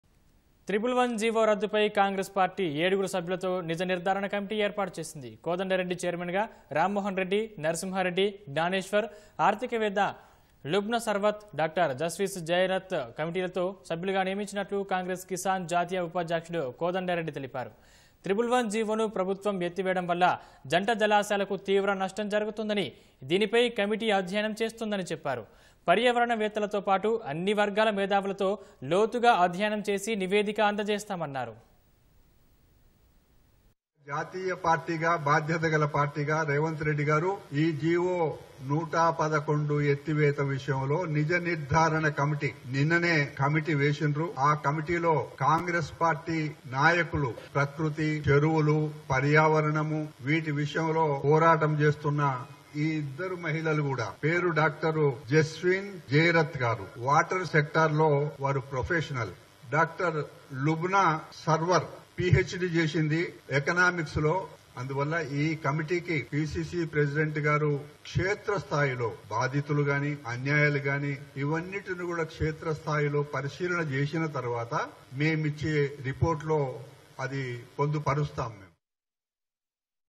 Revoke of GO 111 | Congress Forms Fact Finding Committee | Kodanda Reddy as Chairman With 7 Members
111 జీవో రద్దుపై కాంగ్రెస్ పార్టీ ఏడుగురు సభ్యులతో నిజ నిర్ధారణ కమిటీ ఏర్పాటుచేసింది.  కోదండరెడ్డి చైర్మన్ గా రామ్మోహన్ రెడ్డి, నర్సింహారెడ్డి, జ్ఞానేశ్వర్, ఆర్థికవేత్త లుబ్నసర్వత్ , డా. జస్వీన్ జైరథ్ కలమిటీలో సభ్యులుగా నియమించినట్లు కాంగ్రెస్  కిసాన్ జాతీయ ఉపాధ్యక్షుడు కోదండరెడ్డి తెలిపారు. 111 GOను ప్రభుత్వం ఎత్తివేయడం వల్ల జంట జలాశయాలకు తీవ్ర నష్టం జరుగుతుందనీ, దీనిపై కమిటీ అధ్యయనం చేస్తుందని చెప్పారు. పర్యావరణ వేత్తలతోపాటు, అన్ని వర్గాల మేధావులతో లోతుగా అధ్యయనం చేసి నివేదిక అందజేస్తామన్నారు.